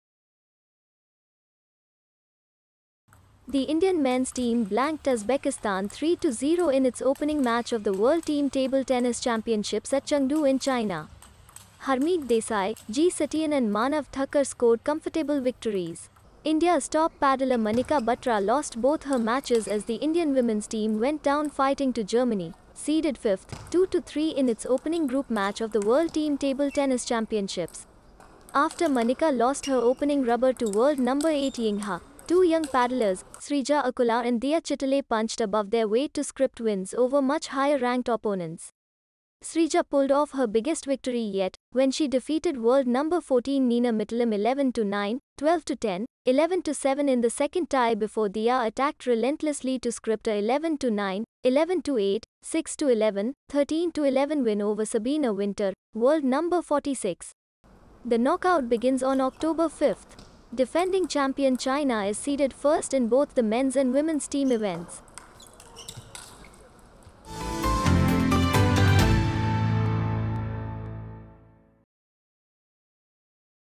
Manika Batra Loses Both Matches, India Beats Uzbekistan
The Indian men’s team blanked Uzbekistan 3-0 in its opening match of the World team table tennis championships at Chengdu in China.